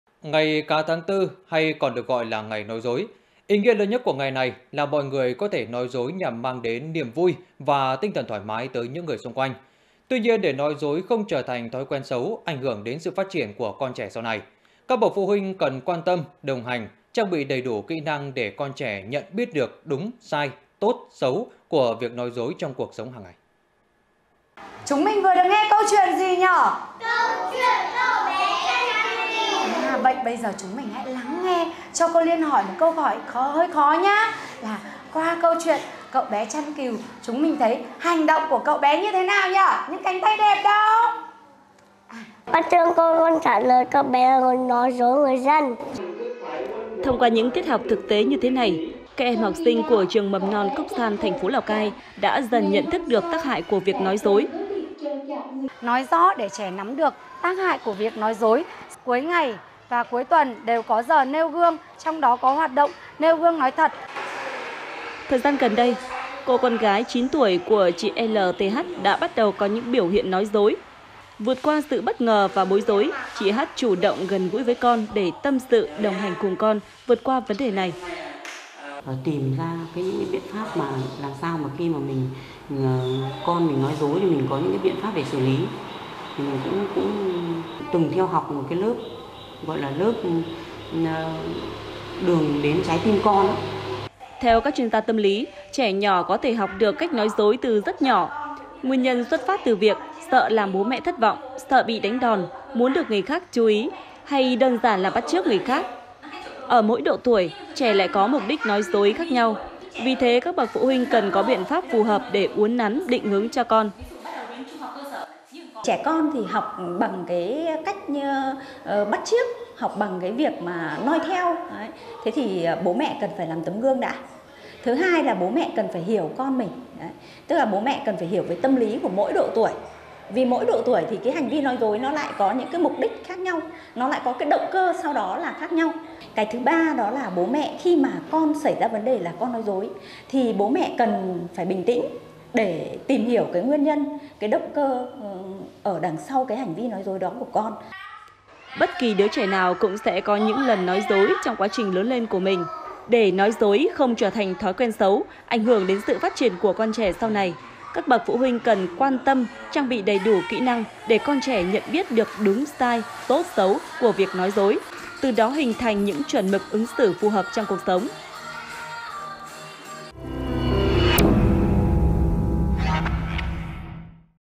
Dạy trẻ nói thật | THLC
Laocaitv.vn - Ngày Cá tháng Tư (1/4) hay còn được gọi là ngày nói dối. Ý nghĩa lớn nhất của ngày này là mọi người có thể nói dối nhằm mang đến niềm vui và tinh thần thoải mái tới những người xung quanh. Tuy nhiên, để nói dối không trở thành thói quen xấu, ảnh hưởng đến sự phát triển của con trẻ sau này, các bậc phụ huynh cần quan tâm, đồng hành, trang bị đầy đủ kĩ năng để con trẻ nhận biết được đúng - sai, tốt - xấu của việc nói dối trong cuộc sống hàng ngày.

Đài Phát thanh - Truyền hình Lào Cai